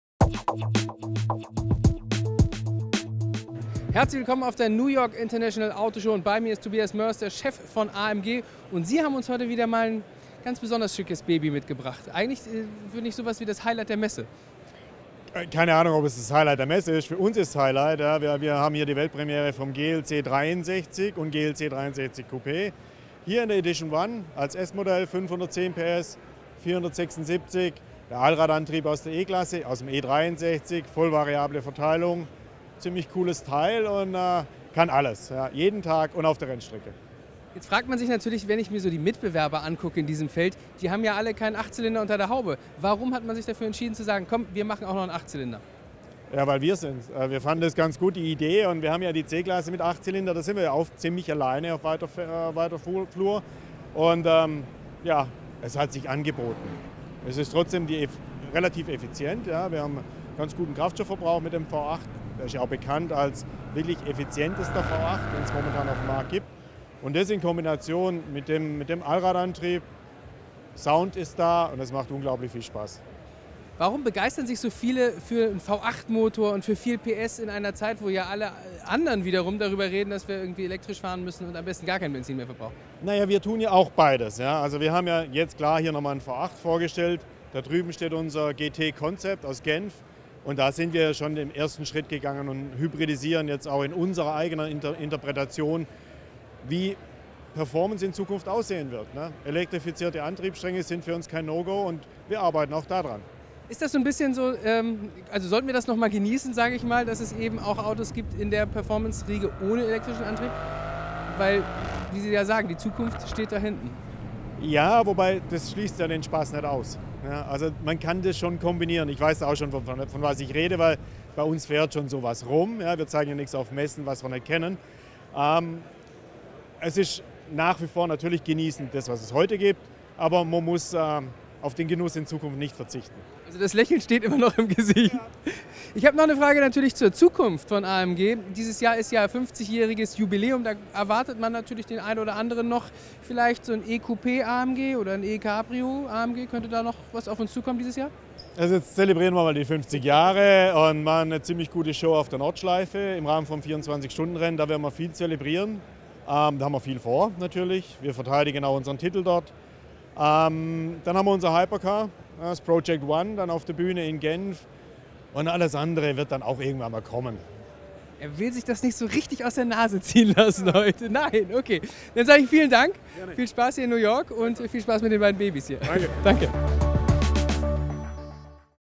Interview zum GLC 63 mit AMG-Chef Tobias Moers (NYIAS 2017)
Auf der New York International Auto Show feiert Mercedes-AMG derzeit die Weltpremiere des GLC 63 und des GLC 63 Coupés. AUTO BILD im Interview mit AMG-Chef Tobias Moers.